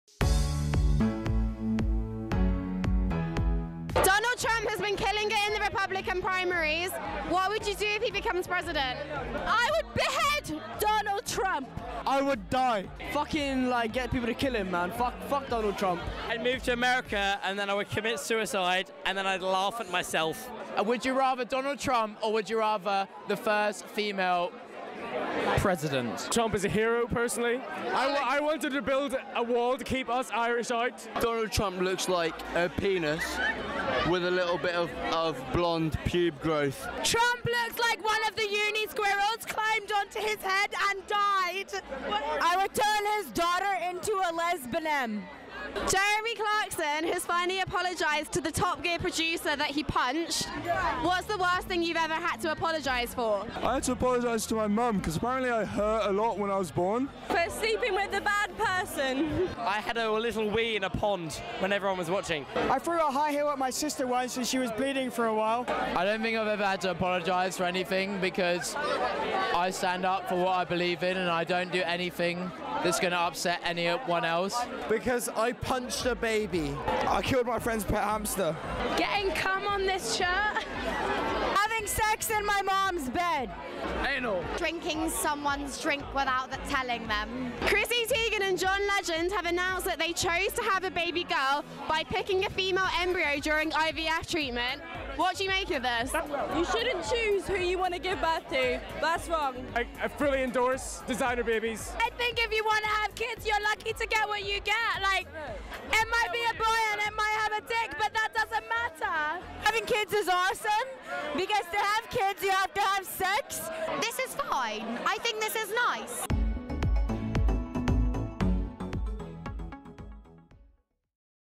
Carpe Noctum News - Wednesday 24th February 2016
This week we are asked about Donald Trump's campaign to become President, John Legend's Designer Baby and Jeremy Clarkson FINALLY apologies!

Want your say? Look out for our CNN Reporters this week, only at CRISIS